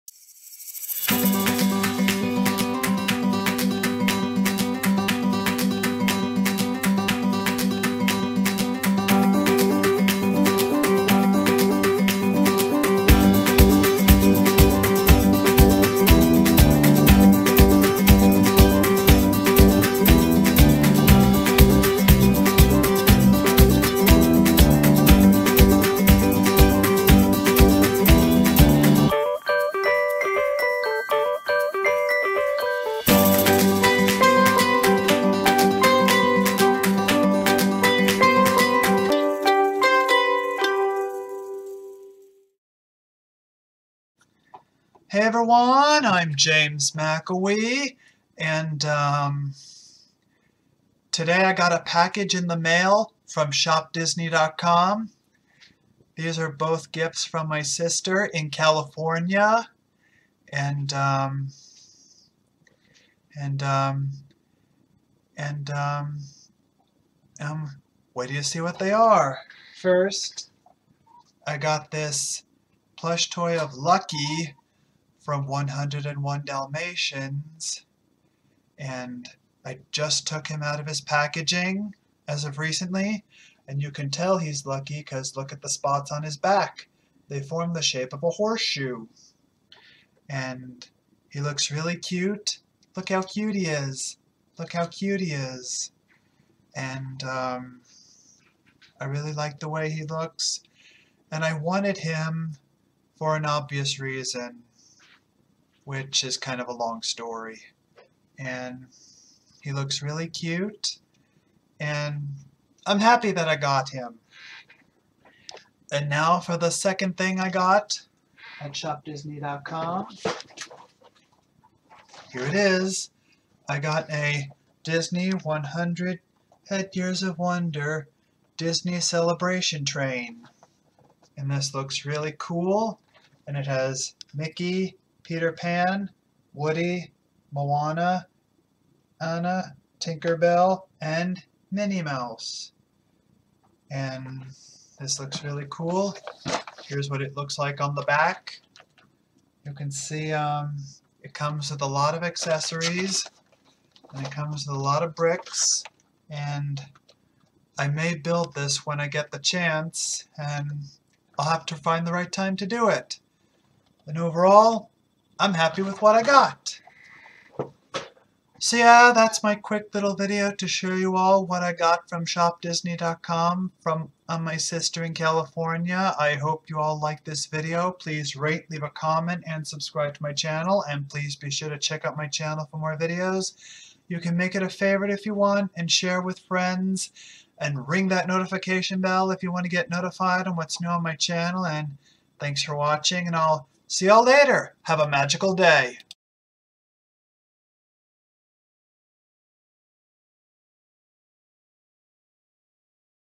Shop Disney Online Haul July 6, 2023
Hey Everyone, I'm James McIlwee.

This is my video of me showing two things my sister in California got for me at Shop Disney Online for my 34th Birthday Gifts.  I hope you like them.